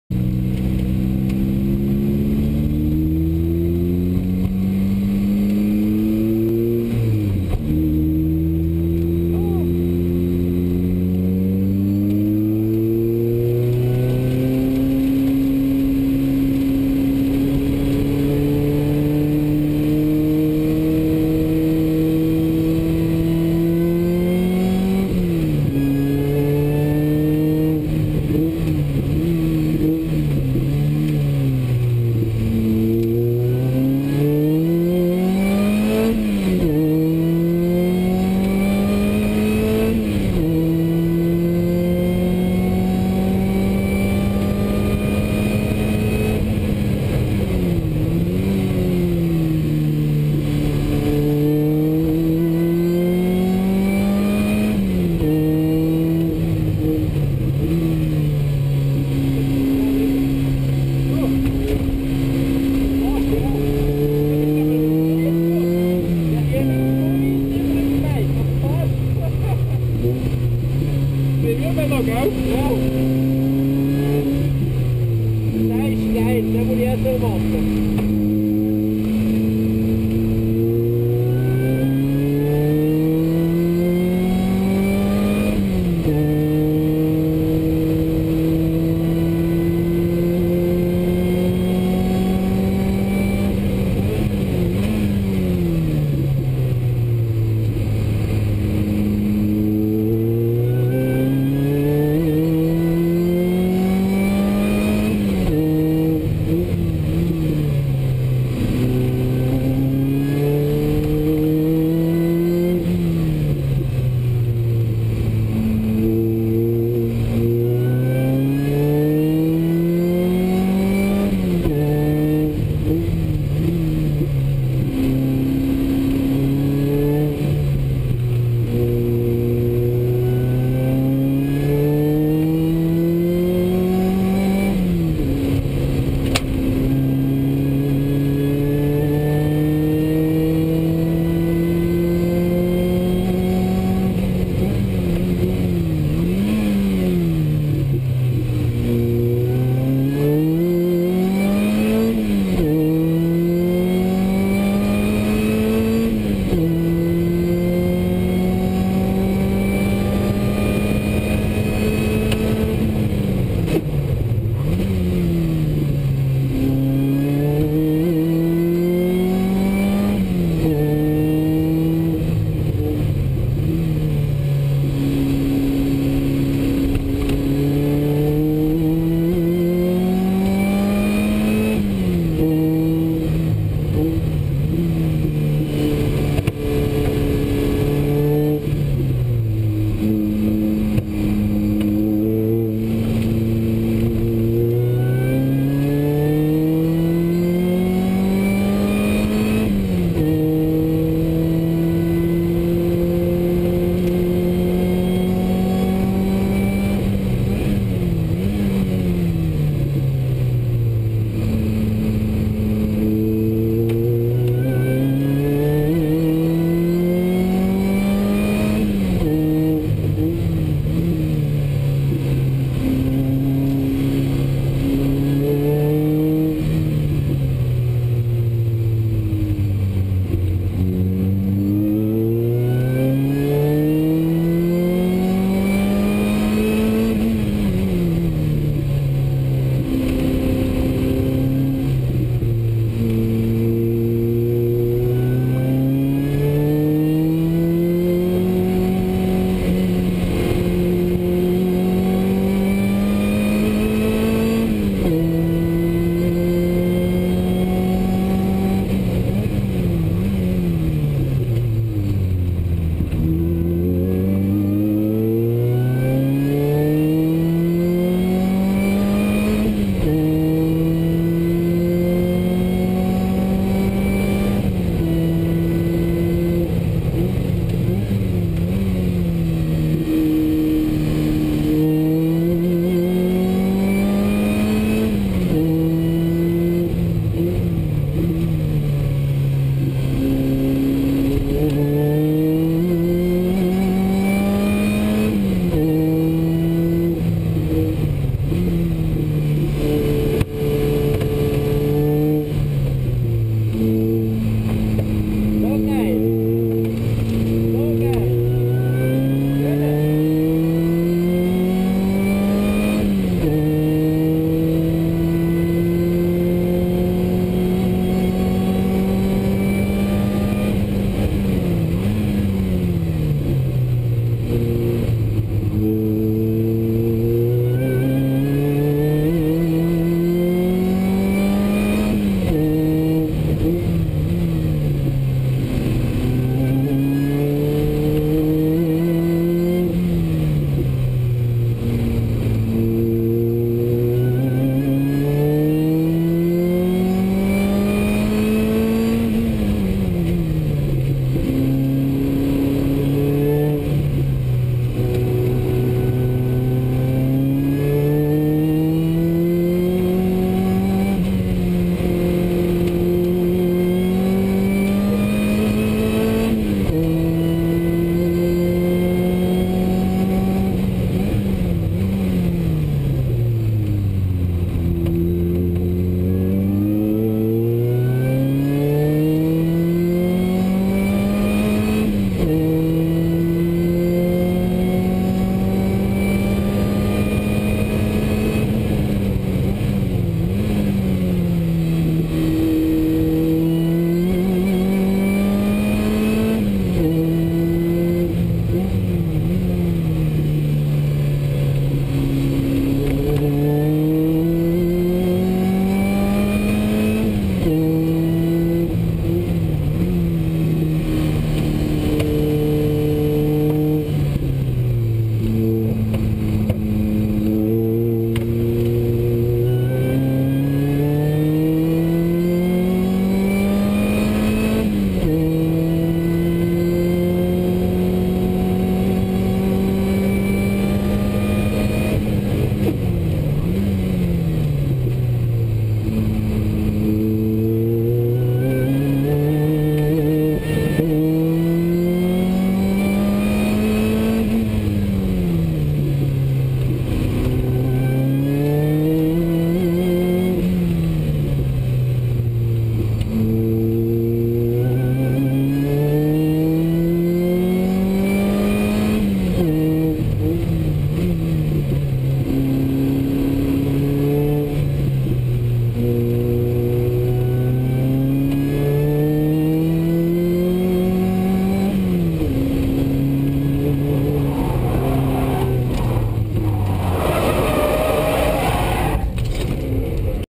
Chenevieres 15.10.2010 Opel Speedster
Opel Speedster Kompressor mit Beifahrer und (zu) schneller Fahrt ;-)